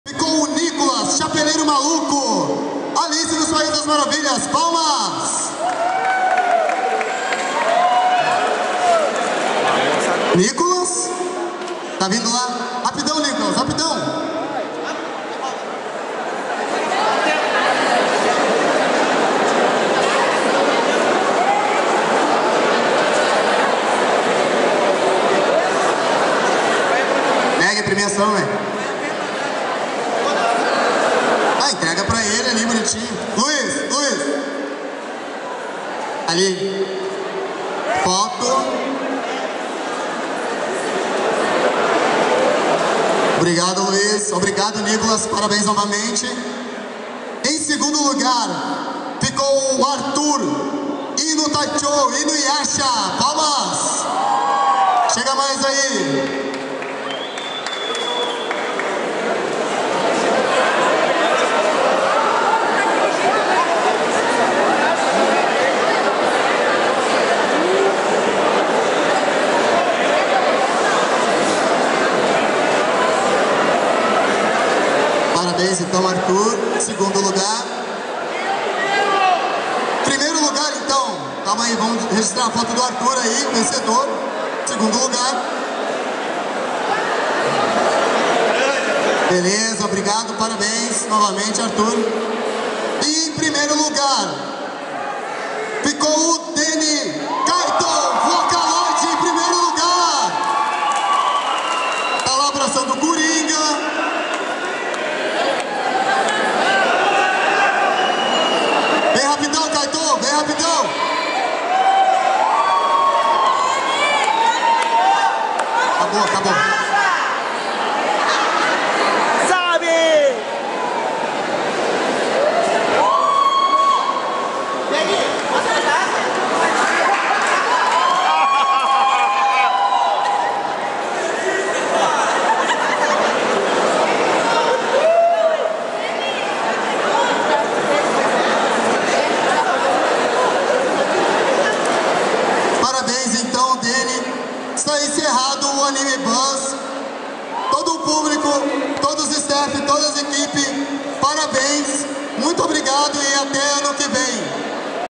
Festival do Japão RS 2016
Participação da Ninja Produtora no Festival do Japão RS 2016